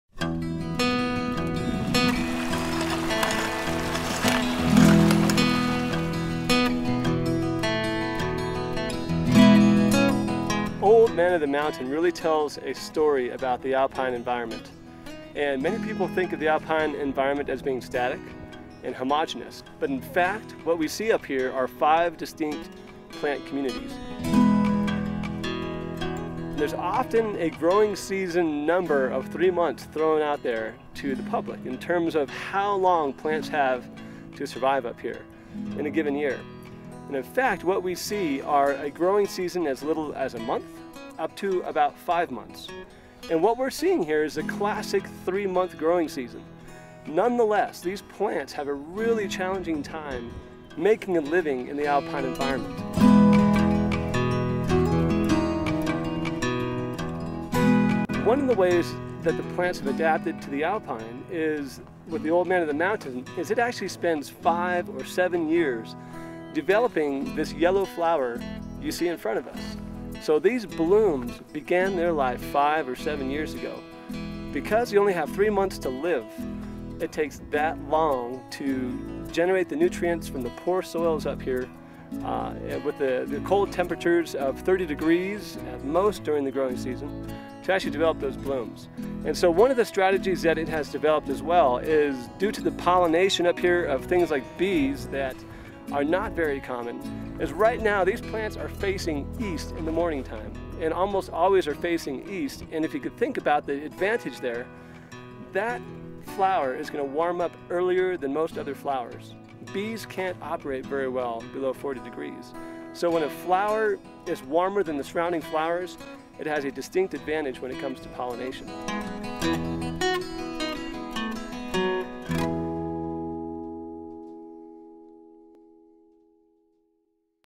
Old Man of the Mountain
Colorado Fourteeners Inititative, is dedicated to preserving and protecting Colorado's 14,000-foot peaks. In this piece, John Giordanengo, an alpine ecologist and former CFI employee, talks about Old Man of the Mountain. This alpine flower has adapted some interesting traits that allow it to thrive in the harsh climatic conditions found on the Fourteeners.